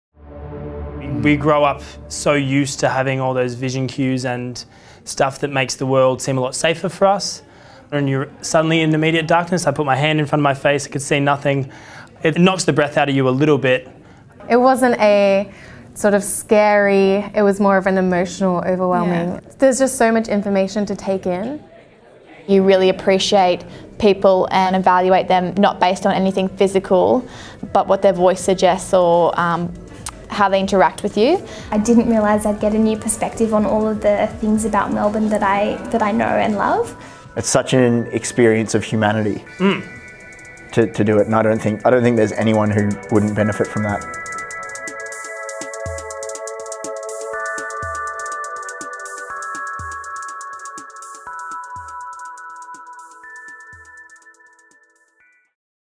Think you've 'seen' it all?
What do the well traveled, tech-savvy generation of today think about Dialogue in the Dark?

"You're suddenly in immediate darkness. It knocks the breath out of you"

Join the global phenomenon and challenge what you know.